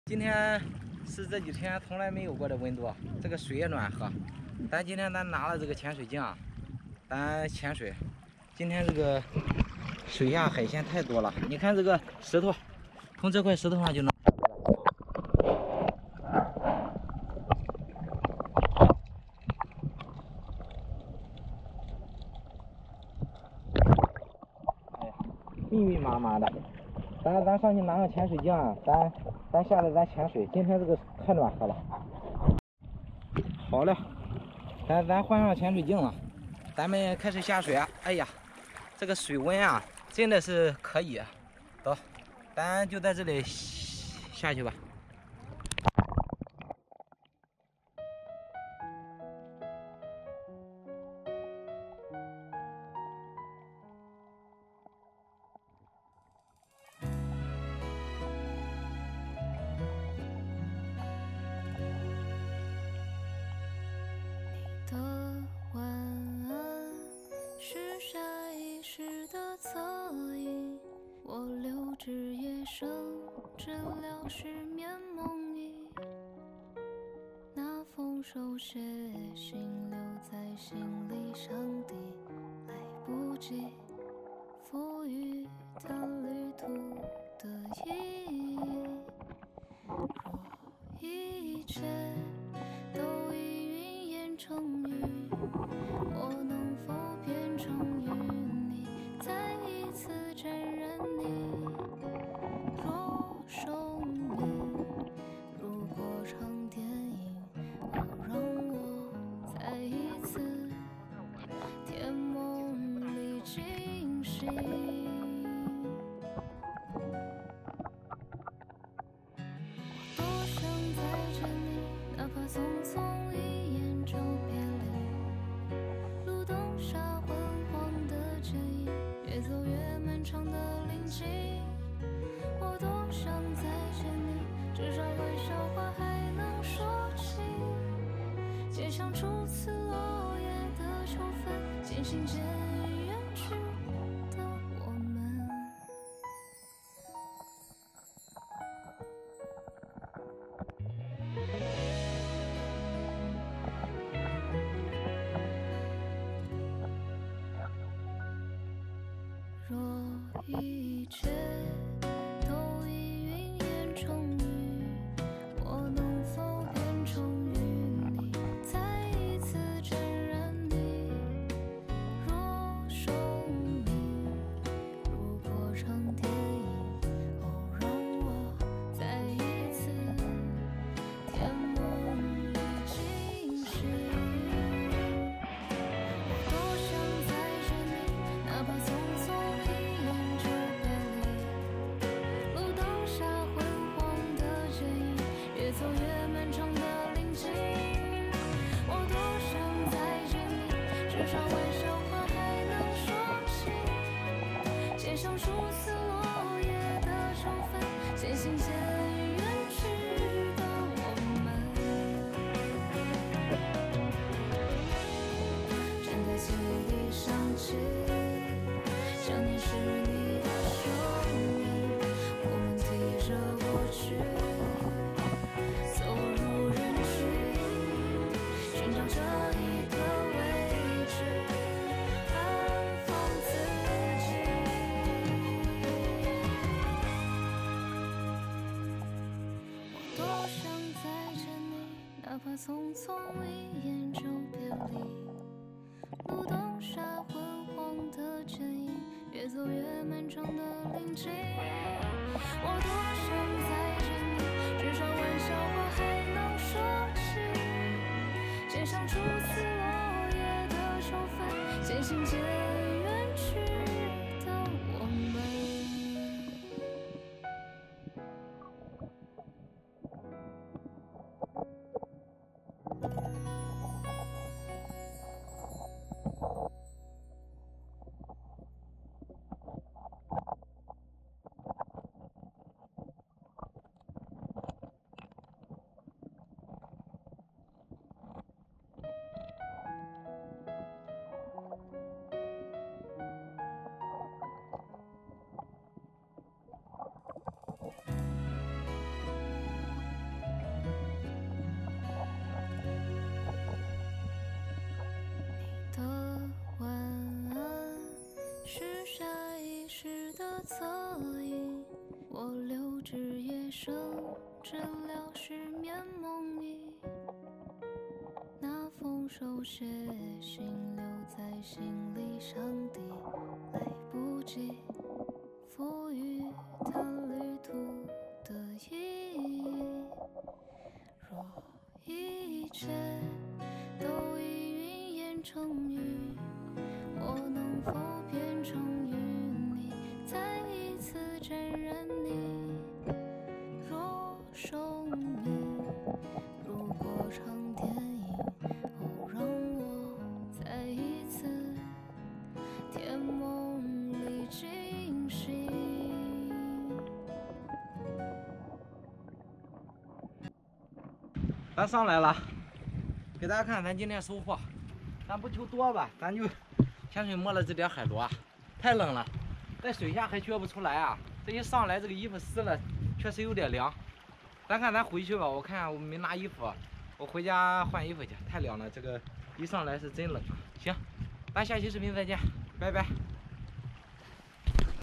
小六趕海潛水抓海鮮，海螺趴在石頭上等著被抓，這下賺大發了【渤海小六】
大家好，我是小六，海螺趴在石頭上等著被抓，這下賺大發了，颱風過後小六去趕海，水下海螺密密麻麻，小六不知該撿一個好了
小六每天在這里和大家分享趕海視頻，分享漁民的海邊生活
小六在這裡與你分享。